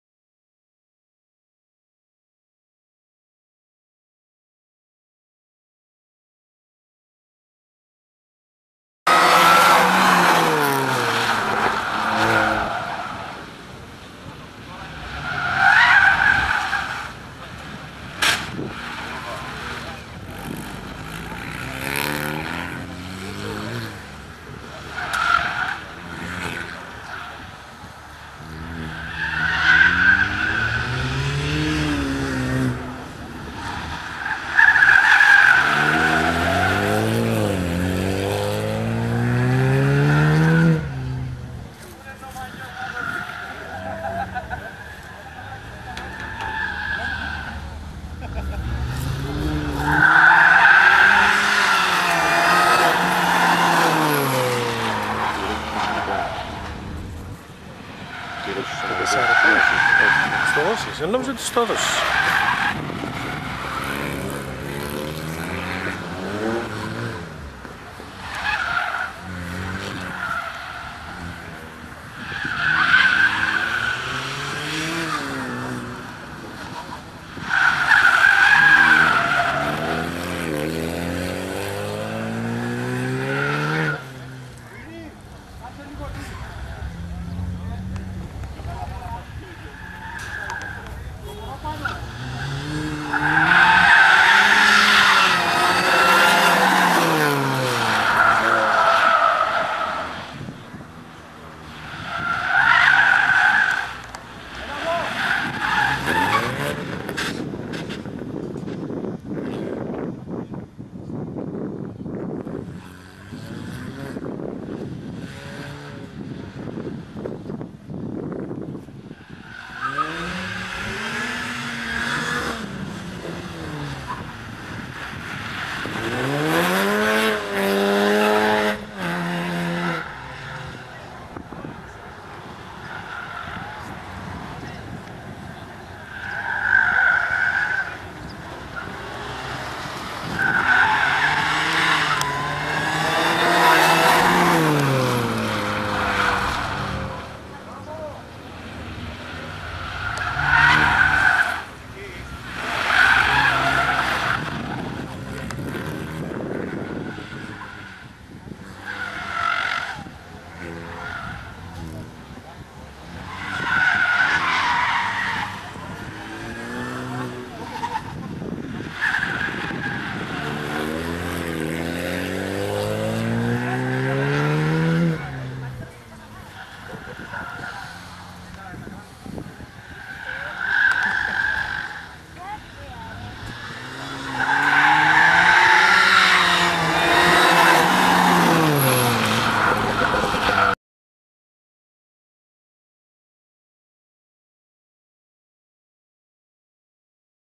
SCOOTER - AUTO & DRIFT RACE 11/12/2011 LARDOS CARTODROME
ΒΙΝΤΕΟ ΑΠΟ ΤΟΝ ΑΓΩΝΑ ΠΟΥ ΕΛΑΒΕ ΜΕΡΟΣ ΣΤΟ ΚΑΡΤΟΔΡΟΜΙΟ ΤΗΣ ΛΑΡΔΟΥ ΣΤΗ ΡΟΔΟ ΣΤΙΣ 11 ΔΕΚΕΜΒΡΙΟΥ 2011, ΜΕ ΠΛΟΥΣΙΟ ΘΕΑΜΑ ! !